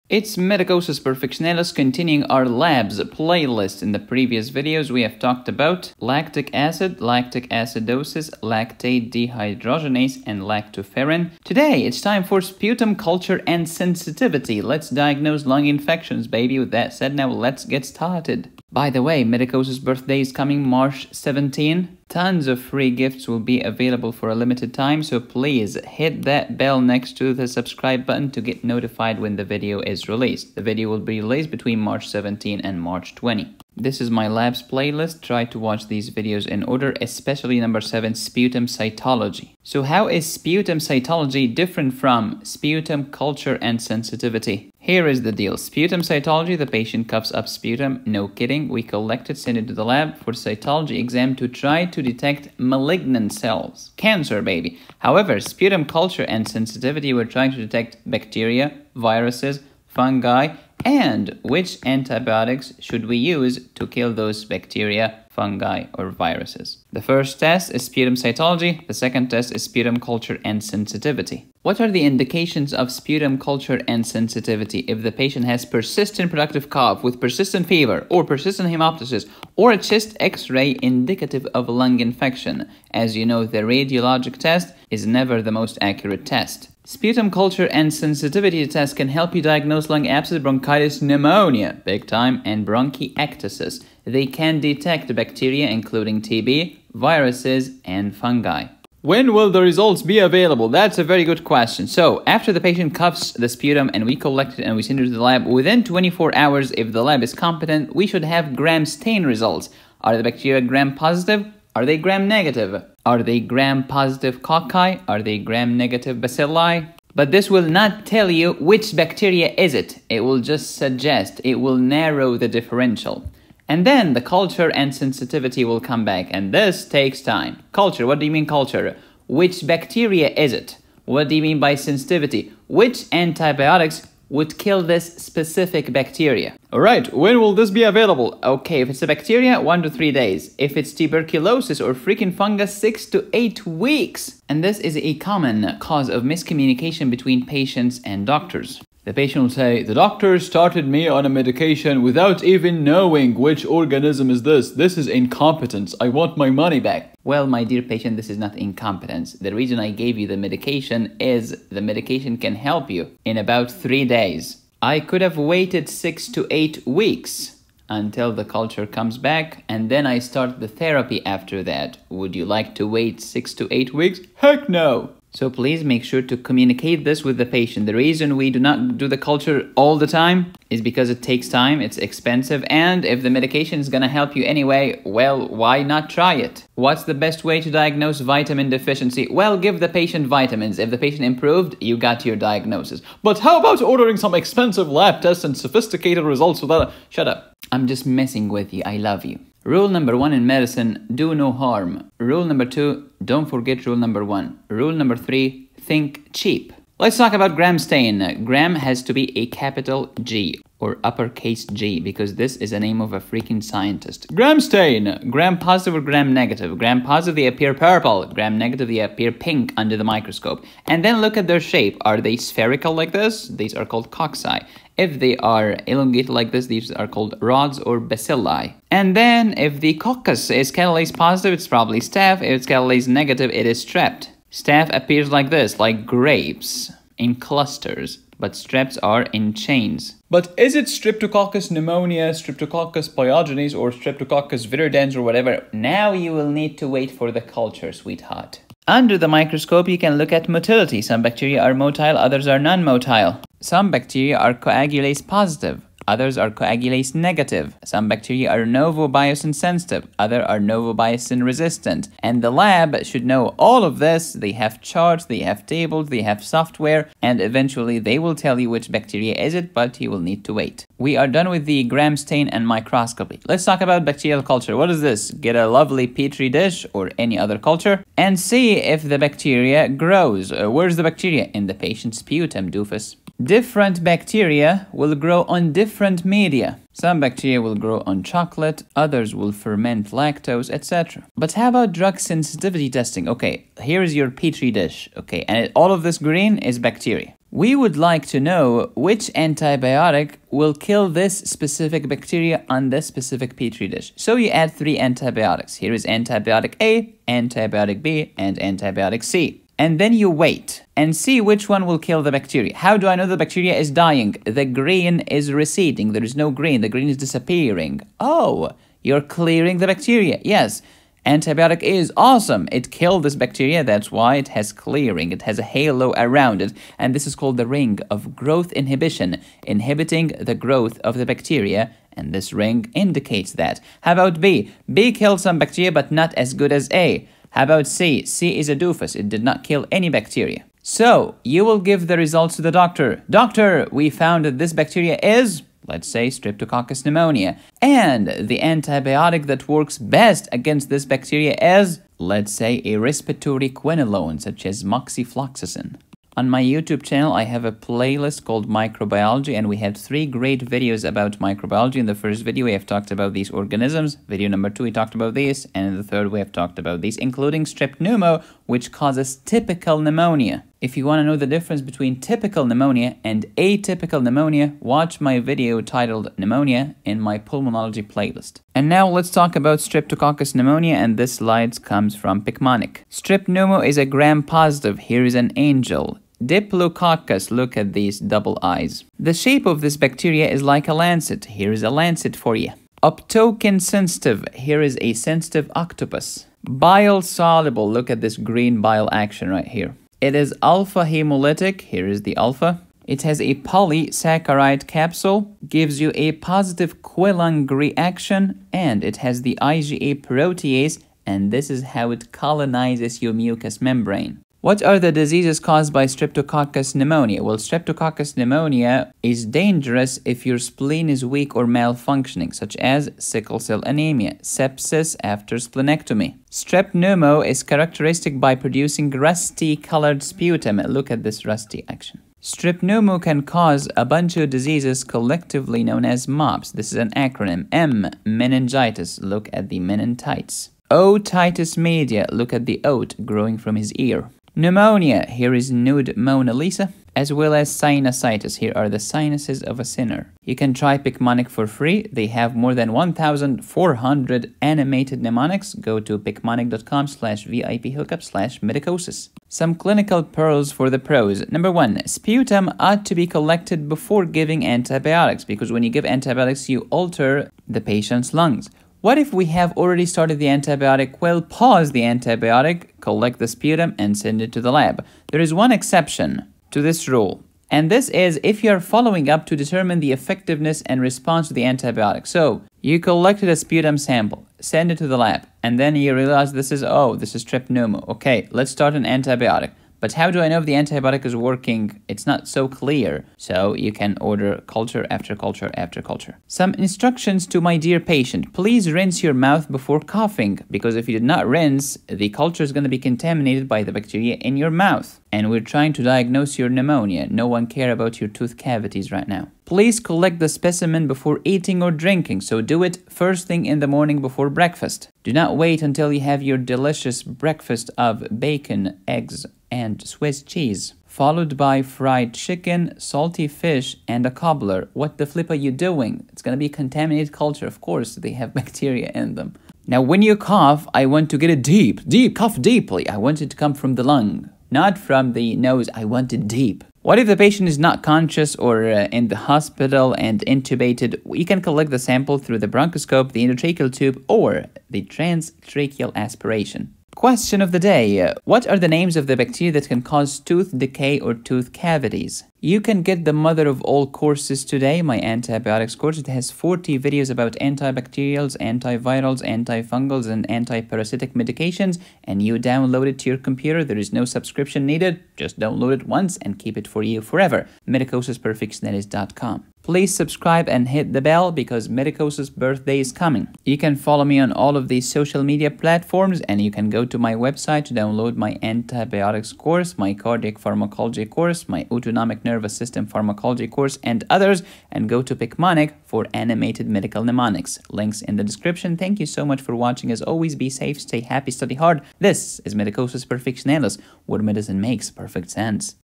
Sputum Culture and Sensitivity - Sputum Sample - Labs 🧪
Sputum Culture and Sensitivity | Sputum Sample | Labs 🧪...Sputum culture and sensitivity can help diagnose pneumonia (lung infection), lung abscess, bronchiectasis, and bronchitis. Sputum cytology is used to diagnose lung cancer. The most common cause of typical community-acquired pneumonia is streptococcus pneumonia.

Happy studying!!